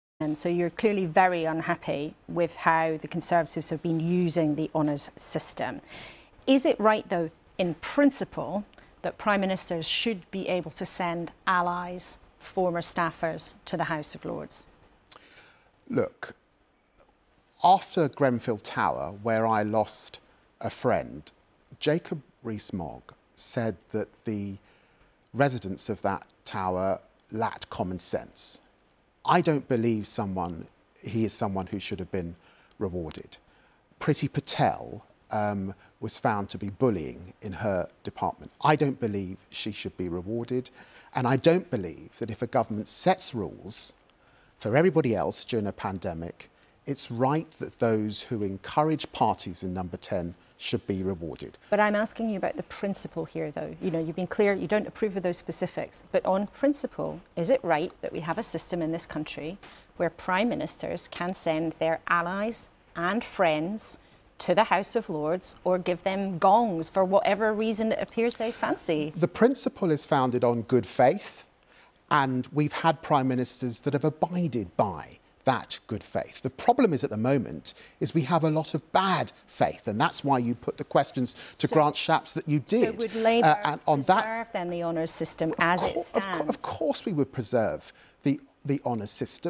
David Lammy - the honours system is operating 'in bad faith'
Laura Keunssberg asks Lammy whether he thinks it is right for the honours system to operate as it does.